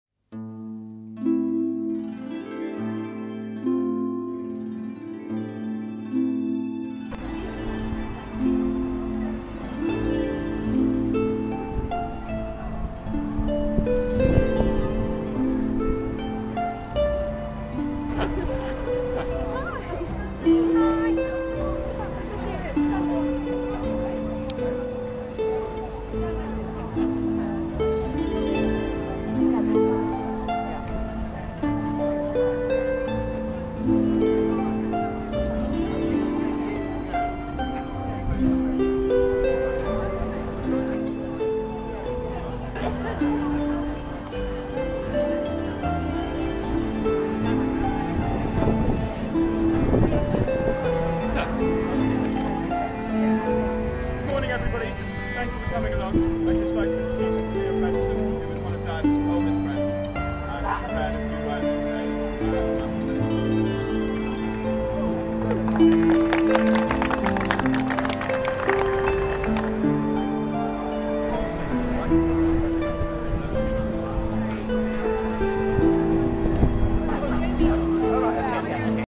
Douglas Bunn Memorial
The Bunn family gather to unveil Philip Blacker's relief sculpture to Douglas Bunn